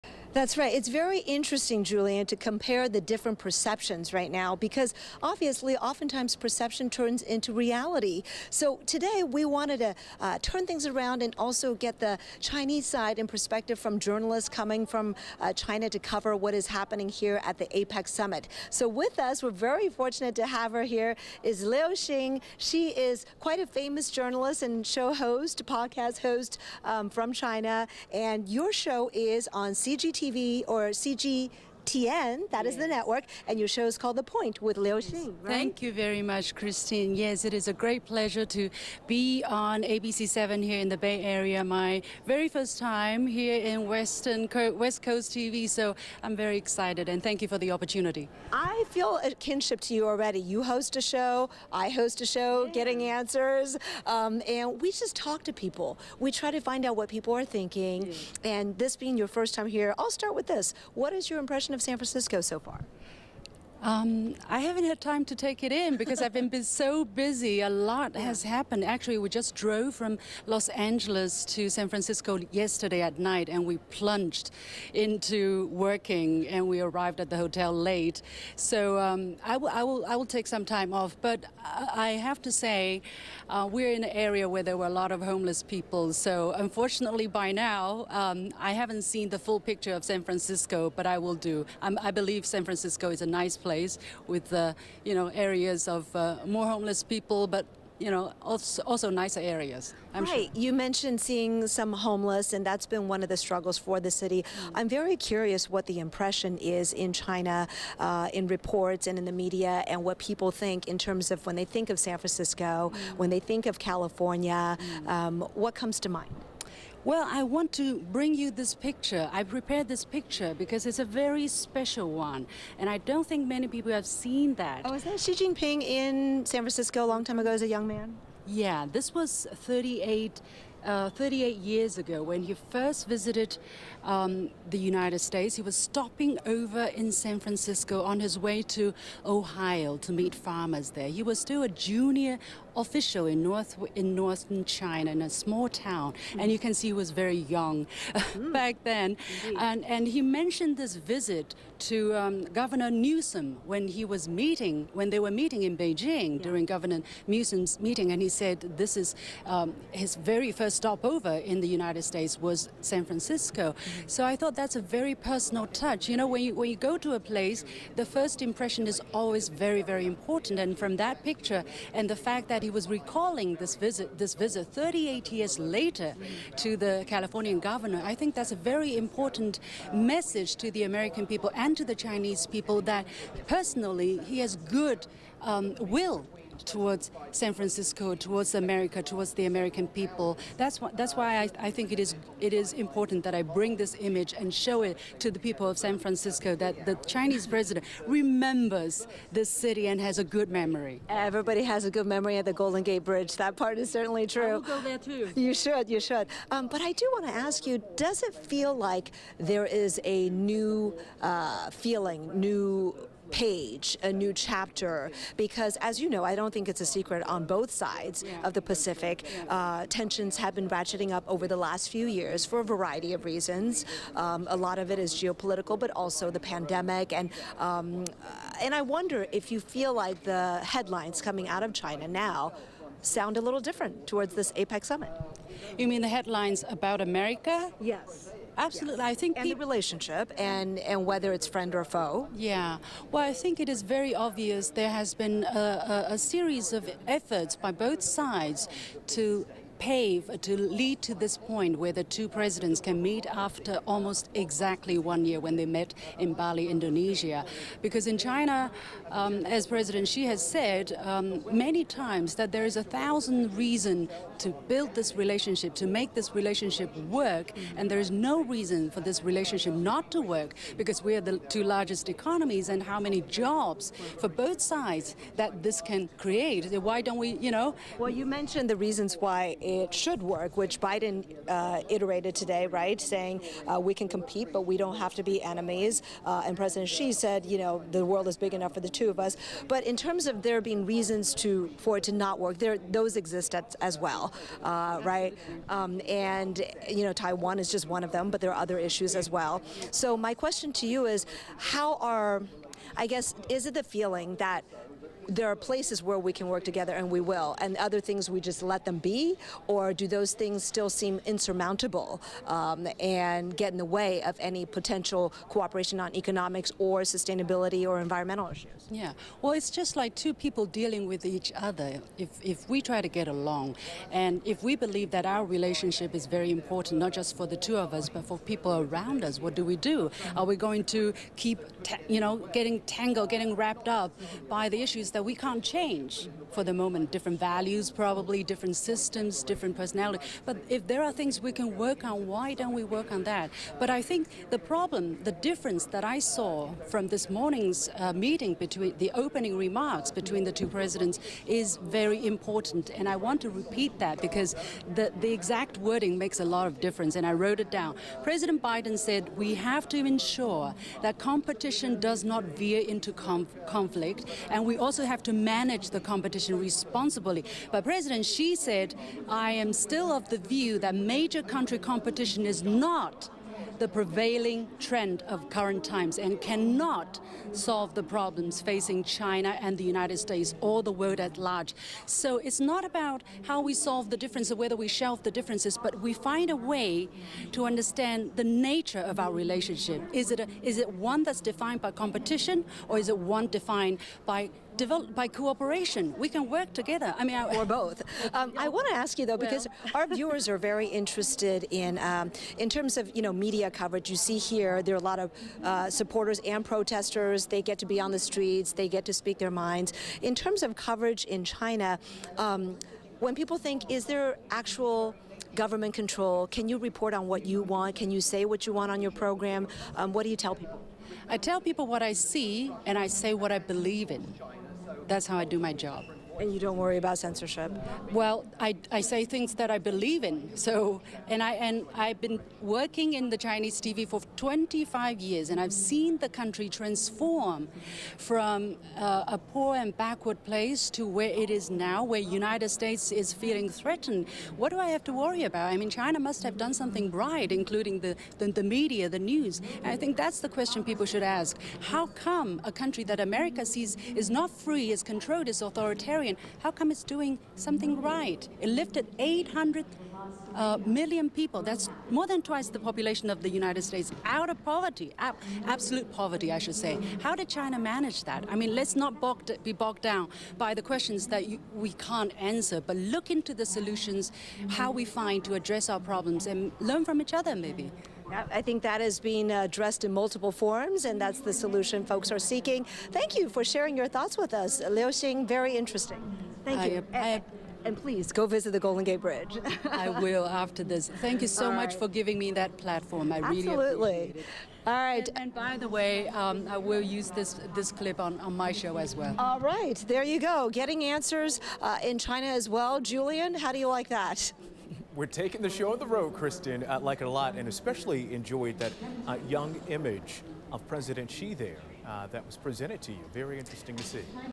Chinese TV host details what Biden-Xi meeting means for China
Chinese television host Liu Xin joined ABC7's "Getting Answers" to talk about what the meeting between President Joe Biden and China's President Xi Jinping means for the country.